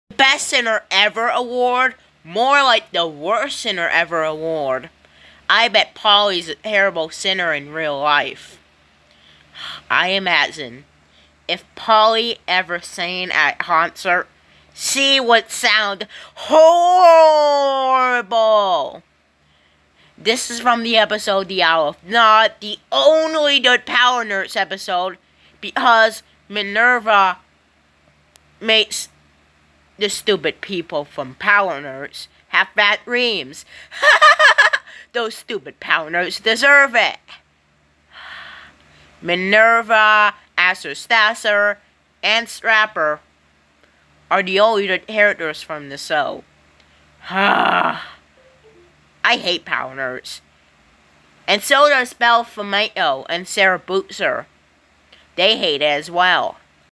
Polly Doesn't Deserve the Best Singer Ever Award. SHE DESERVES THE WORST SINGER EVER AWARD!!!!!!!
I would just imagine one day, when I would be sleeping, I would hear ElsabeakSparkle singing HORRIBLY, so I decided to get several earplugs and earmuffs to block out that stupid ElsabeakSparkle's singing, and I would have a good dream.